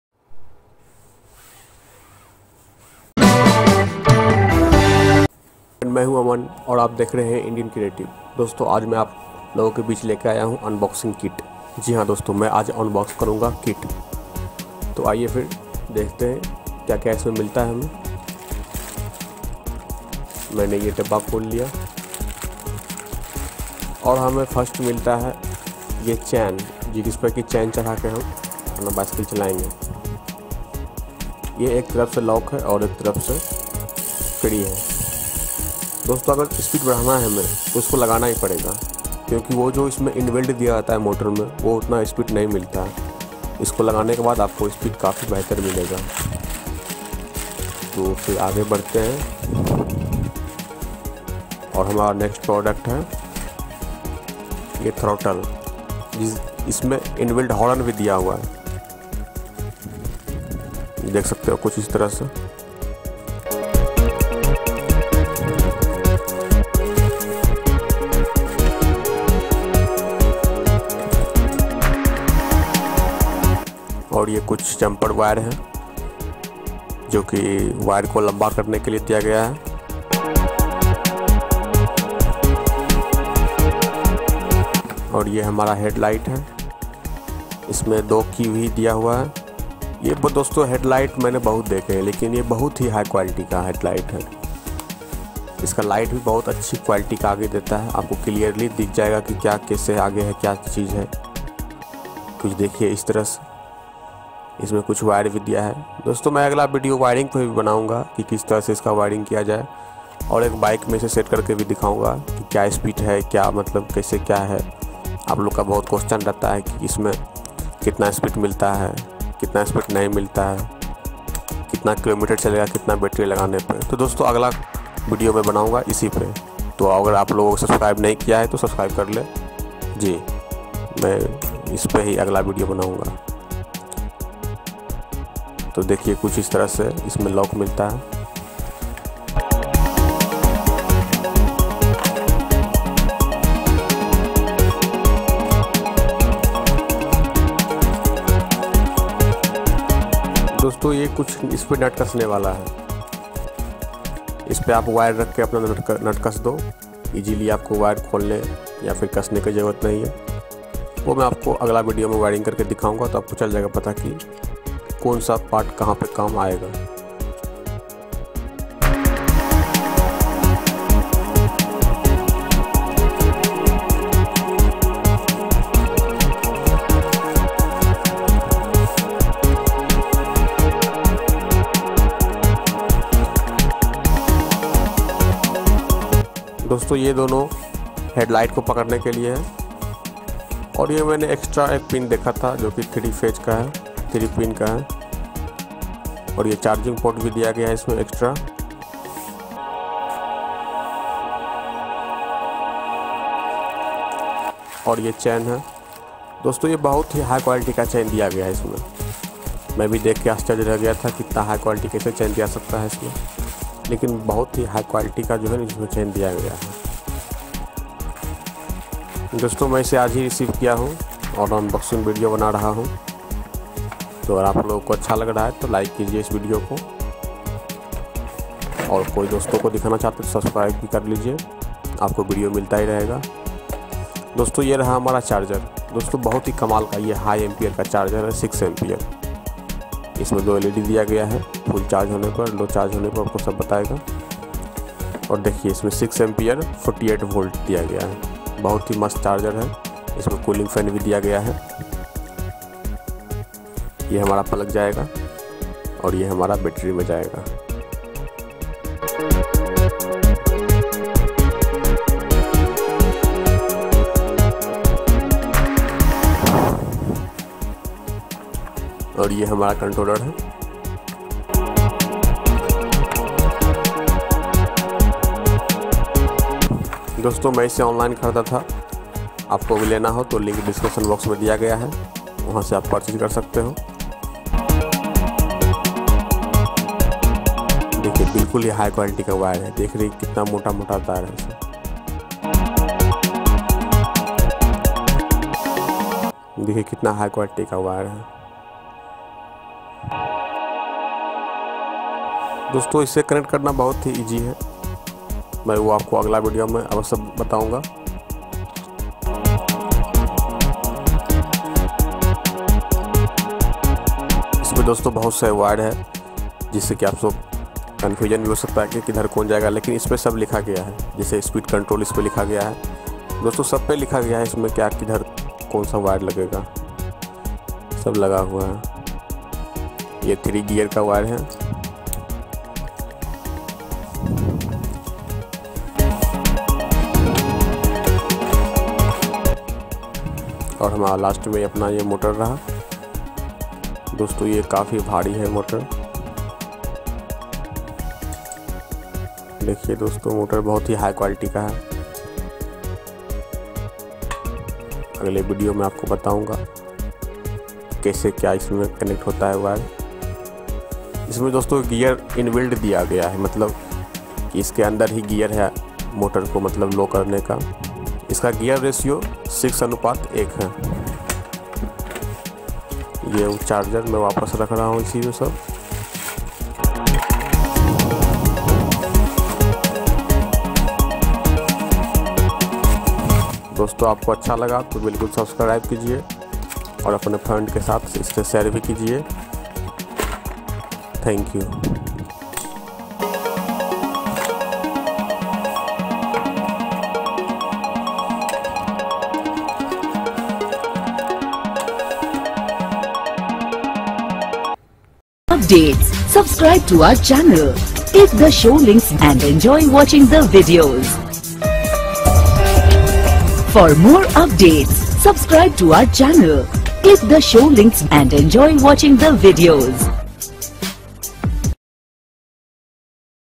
Unboxing e bike 48v 750W BLDC Motor Kit / DIY E bike kit
Hi friend today I understand e bike 48v 750w BLDC motor kit in Hindi language
(11999)

                     Enjoy video friend